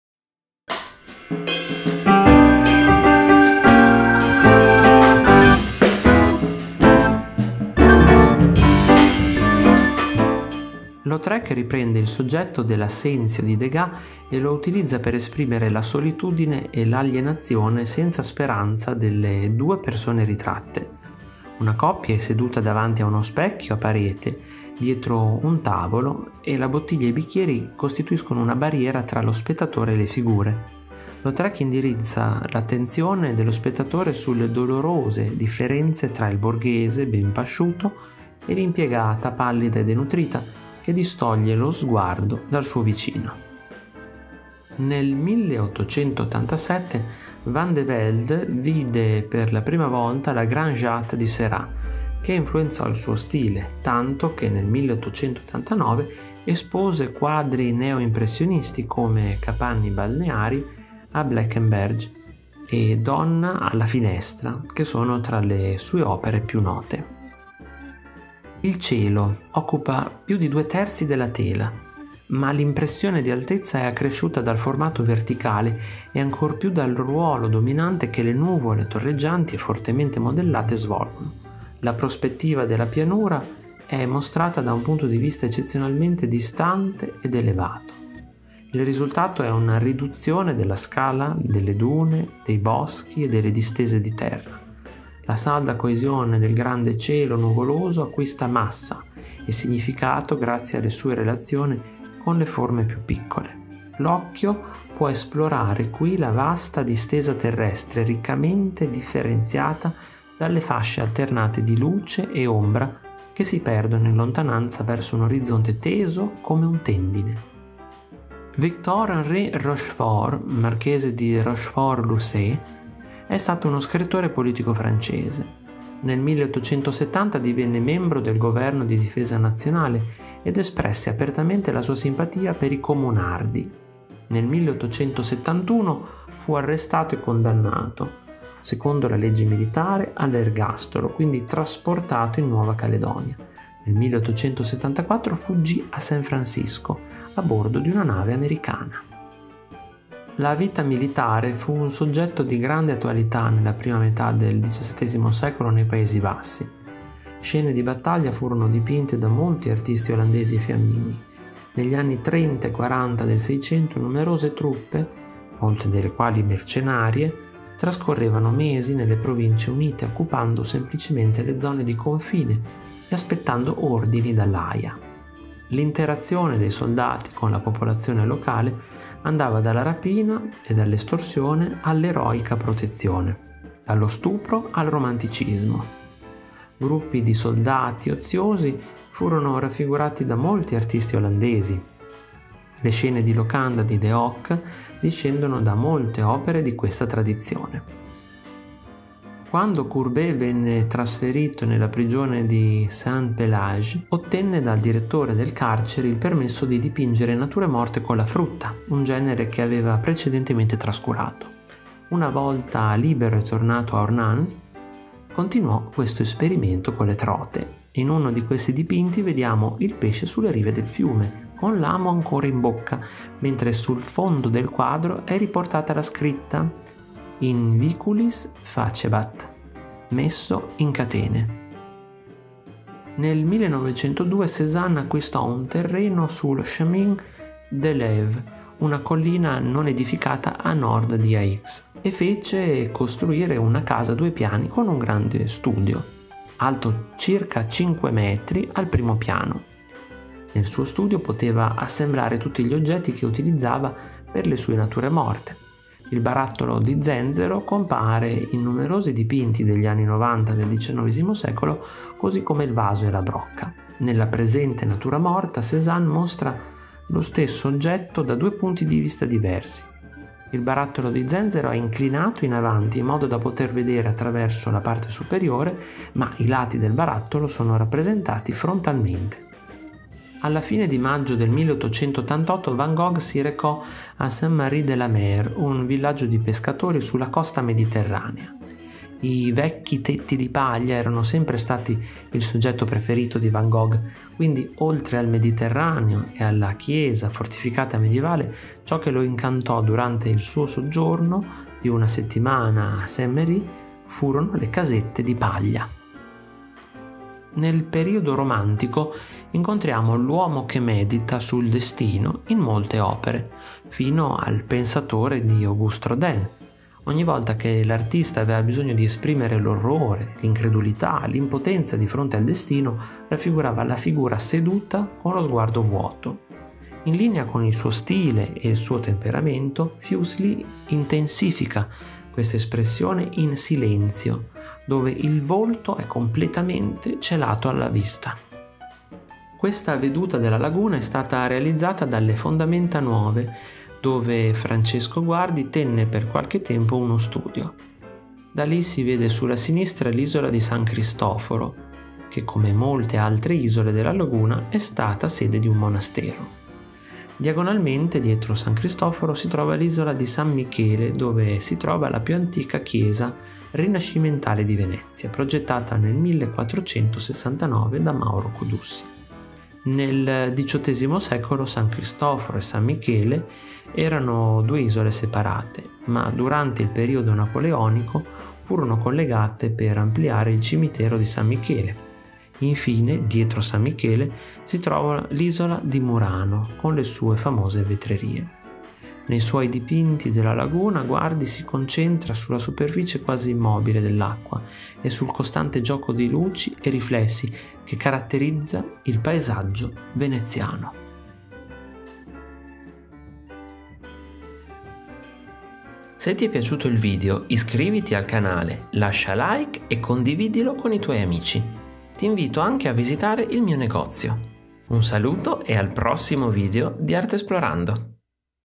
Kunsthaus di Zurigo
Nuovo video della serie curata da Artesplorando, interamente dedicata ai musei e alla loro esplorazione. In questo approfondimento scopriremo 10 opere del Kunsthaus di Zurigo 😄 ▼▼CONTINUA SOTTO▼▼

Il museo possiede opere antiche e medievali, ma la sua collezione consiste soprattutto in pittura, disegni e stampe dal rinascimento in avanti. Gli elementi più considerevoli appartengono all’arte svizzera: Bocklin, Fussli, Giacometti e Hodler sono tra quelli ben rappresentati. Altre opere d’arte di interesse notevole provengono dalla pittura francese dell’Ottocento e nell’arte del XX secolo in generale.